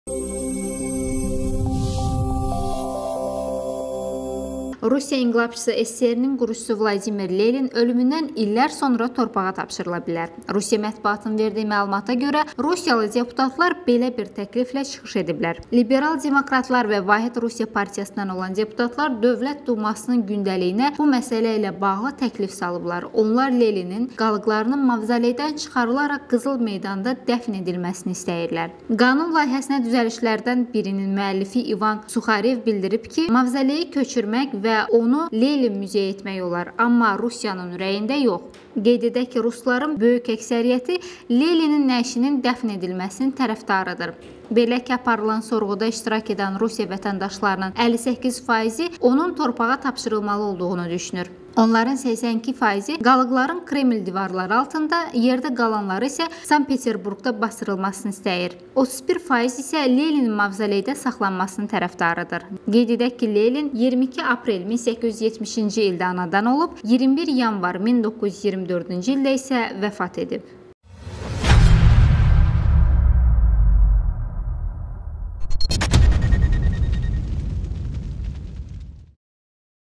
Lenin dəfn ediləcək: "Torpağa verilməlidir" - Rus Pravoslav Kilsəsi onun dəfnində israrlıdır
Rusiya Dövlət Dumasının deputatları Vladimir İliç Leninin qalıqlarının dəfn olunması üçün hüquqi mexanizm nəzərdə tutan qanun layihəsini təqdim edib.
"Russia Today" xəbər verir ki, bu təşəbbüsün müəllifləri Rusiya Liberal Demokrat Partiyasının (RLDP) və "Vahid Rusiya" Partiyasının deputatları olub.
Deputatların fikrincə, sənəd tarixi şəxsiyyətlərin qalıqlarınin dəfn edilməsinə mane olan hüquqi boşluğu doldurmaq üçün nəzərdə tutulub və bununla Leninlə bağlı işə son qoyulacaq.
Qanun layihəsinin təşəbbüskarları Rus Pravoslav Kilsəsinin mövqeyinə əsaslanır. Belə ki, kilsə onun qalıqlarının torpağa verilməsinin, yəni dəfn edilməsinin zəruri olduğunu bildirir.
Qeyd olunur ki, layihə aprelin 22-də dünyaya gəlmiş V.İ.Leninin doğum gününə həsr olunub